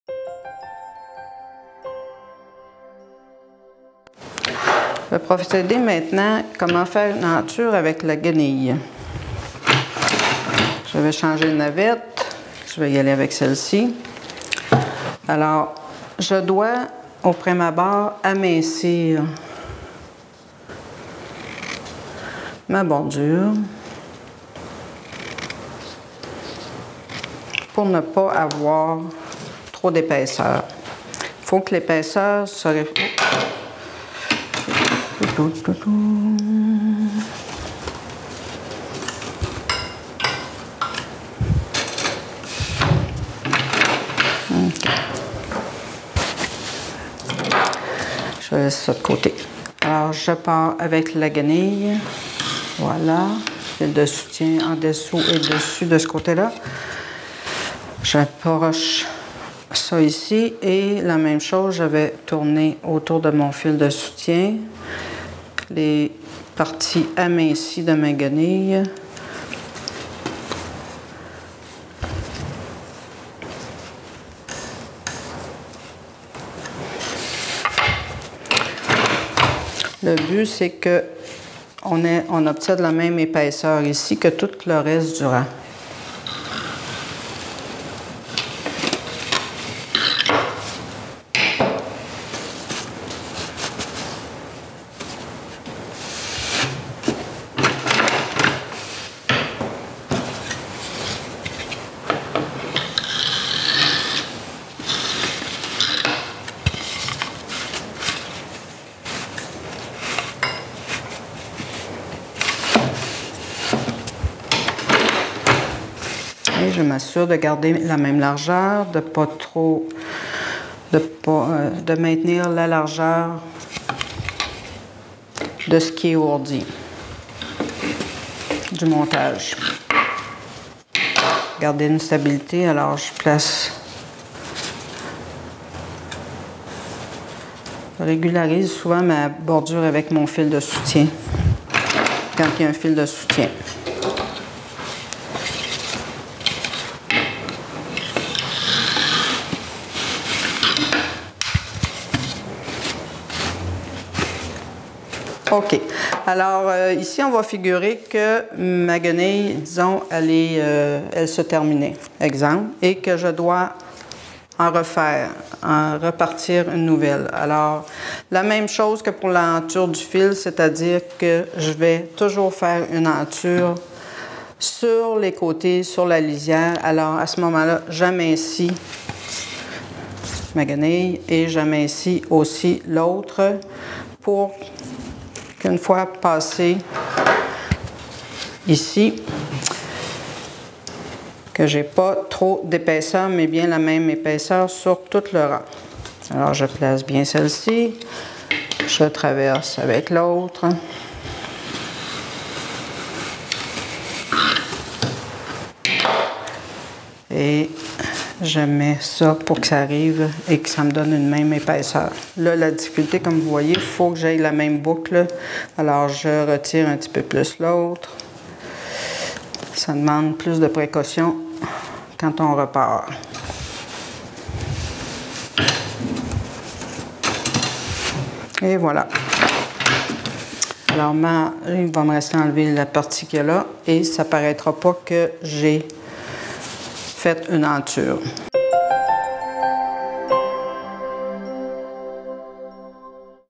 Tissage – Comment faire de belles lisières et entures – partie 2
Poursuivez votre apprentissage avec les conseils méthodiques de Monique Carrier, Fière d'être Fermière. Comment bien utiliser la guenille.